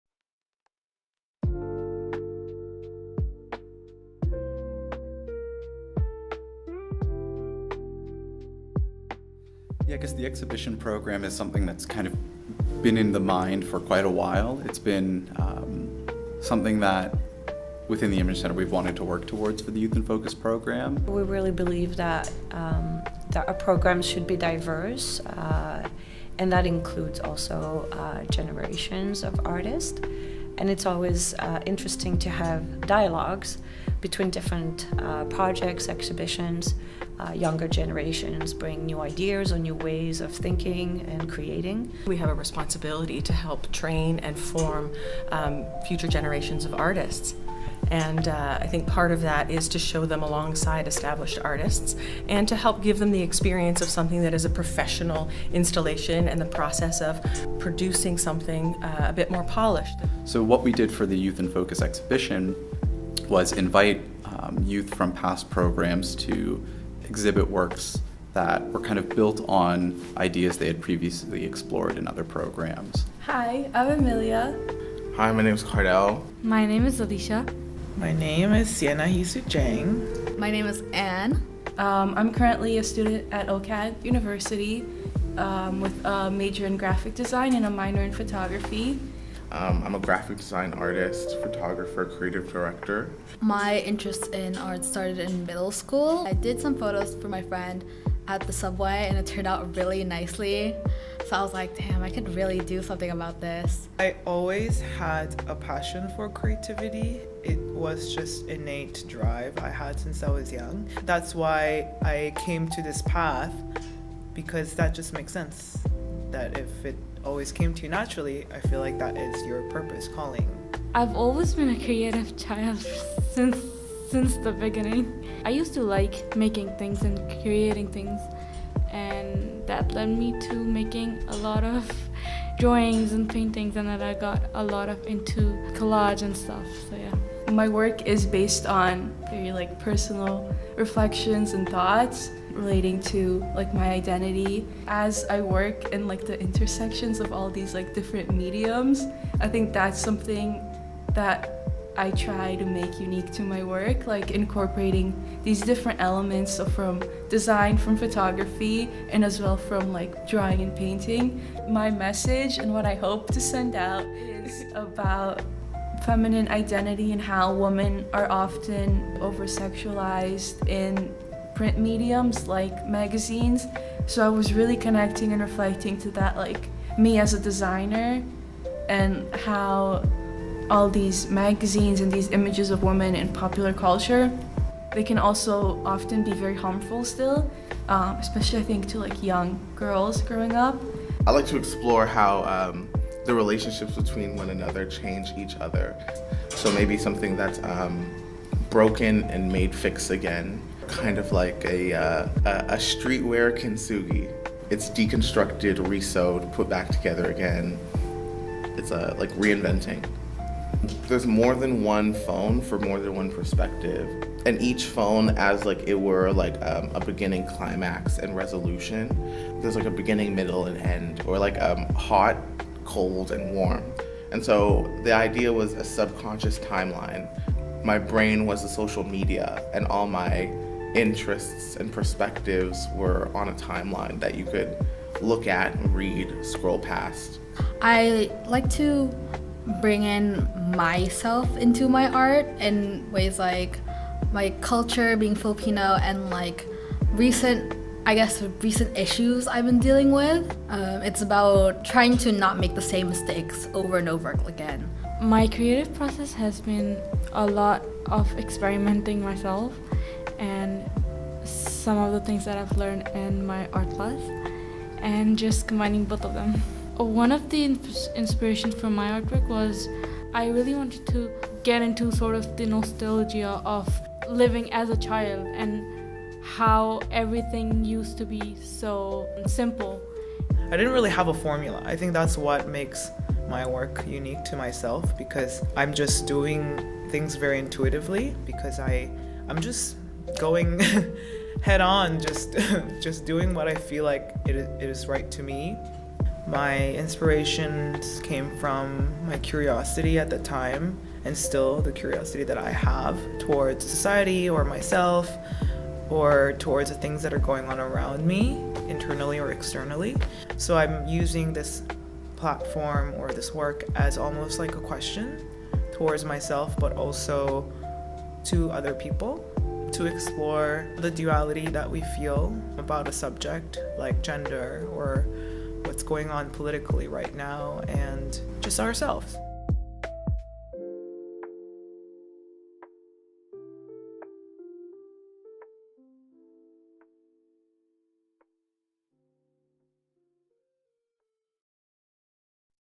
In Dimension: Personal and Collective Narratives
In Dimension: Personal and Collective Narratives is a group exhibition bringing together works by emerging artists who participated in The Image Centre’s Poy Family Youth in Focus Program. This program provides free, customized workshops for community organizations and youth in the Toronto area, offering hands-on photography-based activities that encourage critical thinking about the IMC’s exhibitions and promote explorations of image-making and storytelling processes.

In Dimension features the works of Alishah Ahmad, Anne Balneg, Siena-heesoo Jang, Kardell McAfee, and Emiliya Volchenko, who participated in the IMC’s recent workshops “Representation as You” (2021) and “Refaced: Photography and Collage” (2023). The artists use diverse media to find common ground in autobiographical methods of artmaking, emphasizing self-reflection and exploring their singular understandings of popular culture, nostalgia, social structures, and personal emotions. Through a combination of photography, painting, illustration, and sculptural techniques, and using imagery appropriated from magazine spreads, antique photo albums, and social media, these works construct a connection between individual and collective narratives.

Contributing to the creative discourse at The Image Centre, this new generation of artists attests to the significance of lived experience, and exemplifies self-exploration as a creative process.

Guest Curators: Bryce Julien, assisted by Campbell Kaye

This exhibition is supported through the generosity of the Poy Family.